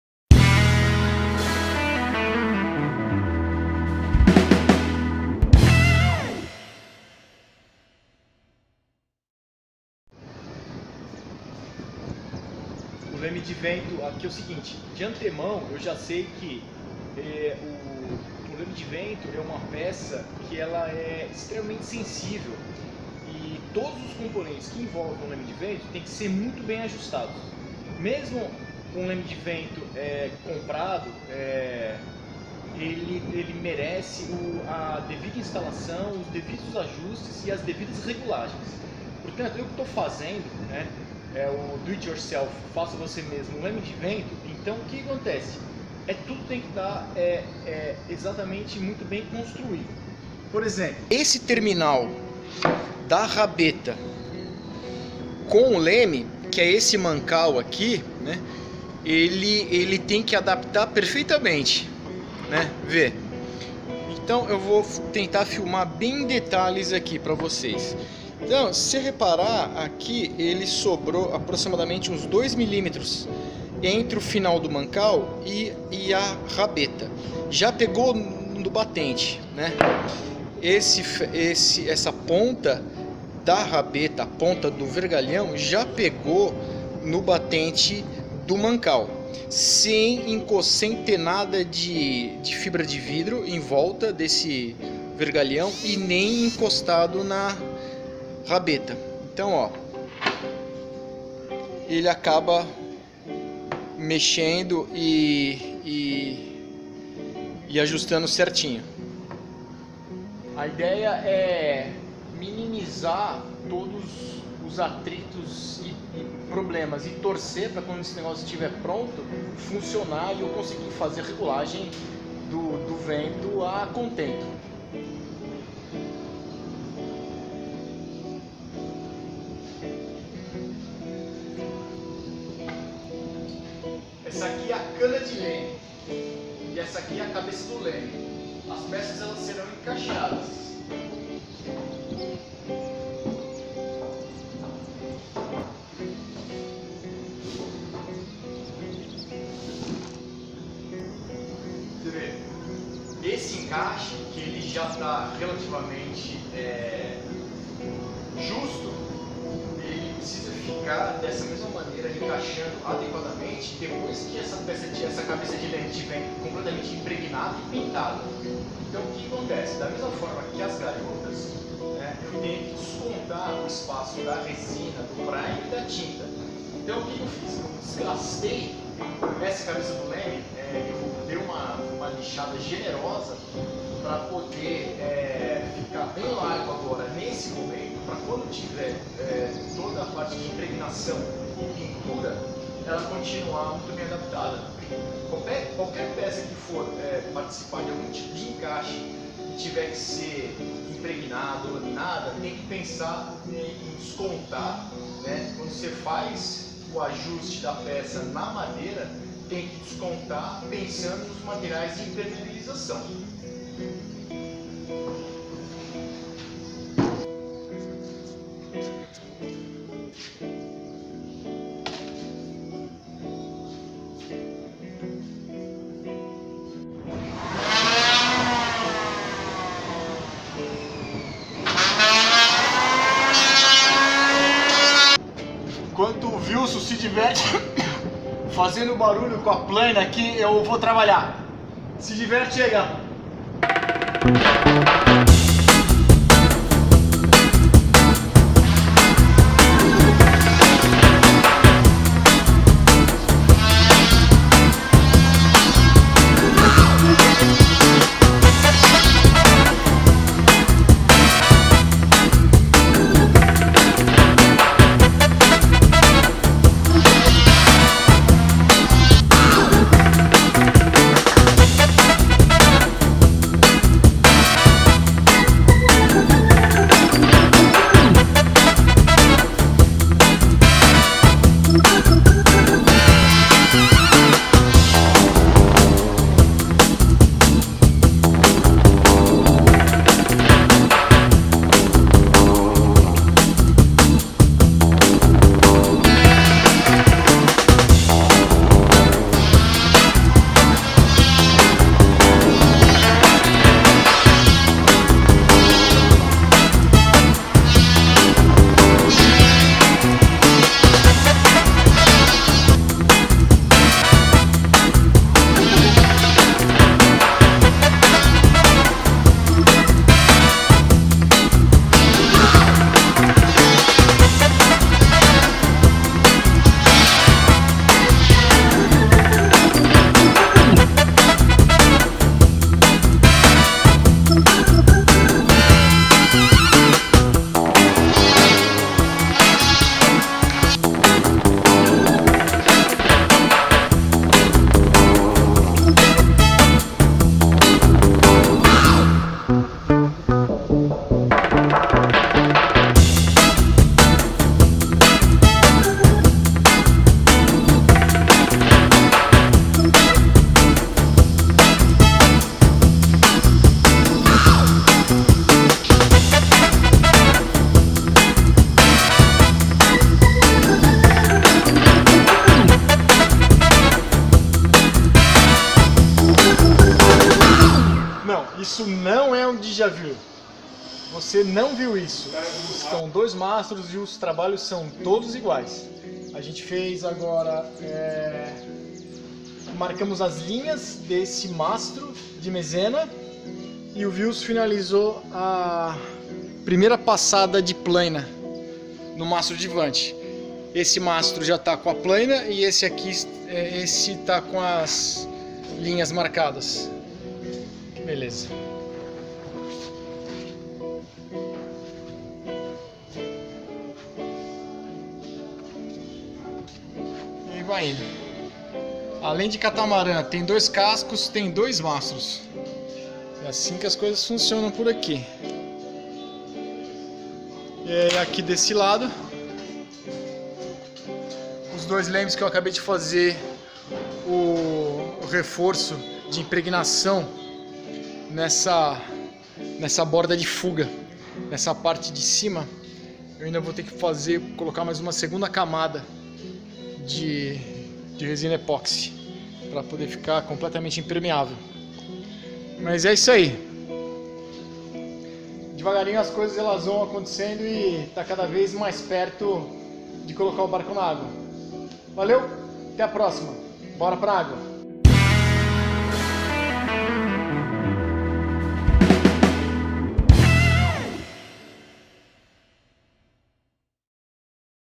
Leme de Vento A Saga Continua Ep 40
Continuo na impregnação do leme e da rabeta do leme de vento, peça  que vai a ré do leme e que vai ser o responsável por mudar o rumo do barco quando o vento mudar.